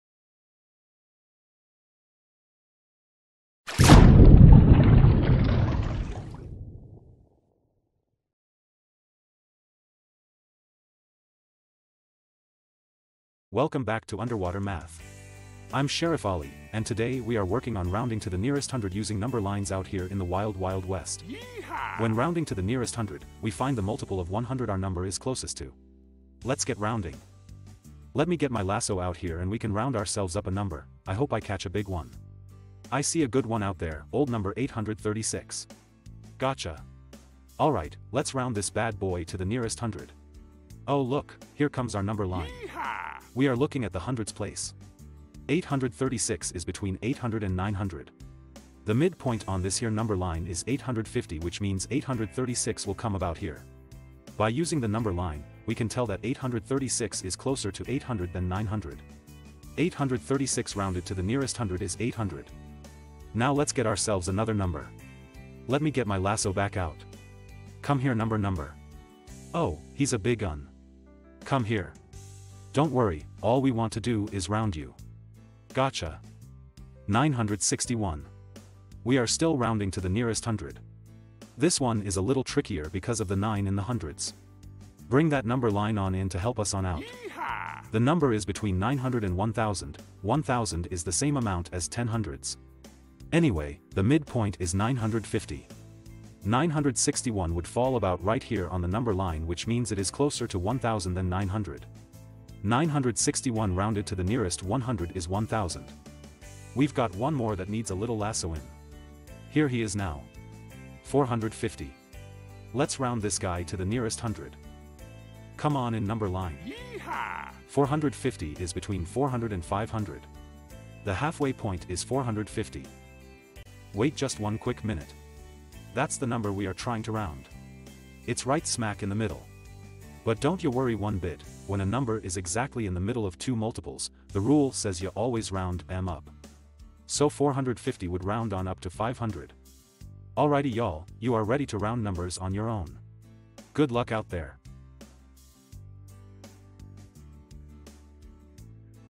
Round Numbers up to 1,000 to the Nearest 100 Using Number Lines | 3rd Grade Math
Underwater Math provides engaging learning solutions for students. This video helps students round numbers to the nearest 100 using number line supports. Georgia Standard NR.1.3